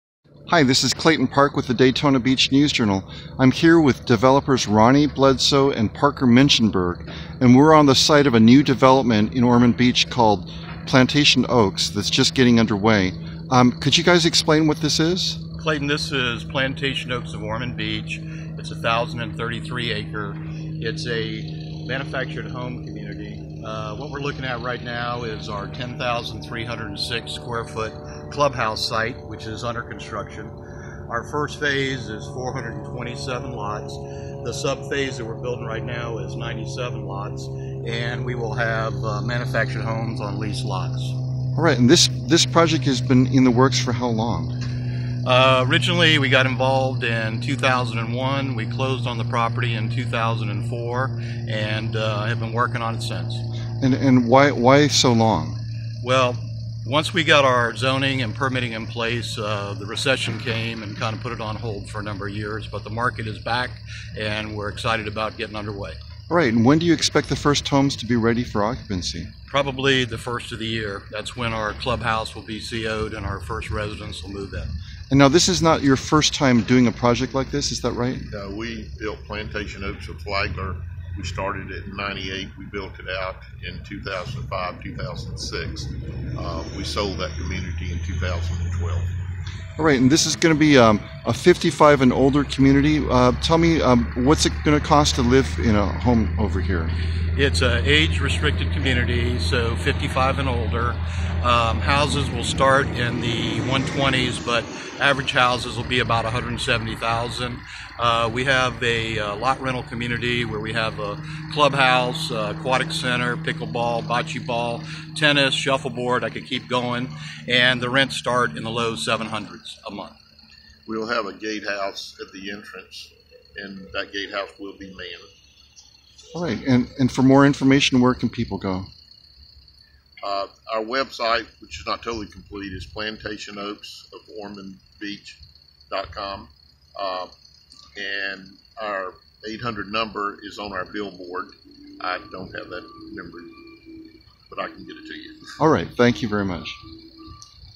Developers Ronnie Bledsoe and Parker Mynchenberg talk about their Plantation Oaks of Ormond Beach pr
Developers Ronnie Bledsoe and Parker Mynchenberg talk about their Plantation Oaks of Ormond Beach project

By: claytonpark3700
Published on: June 18, 2017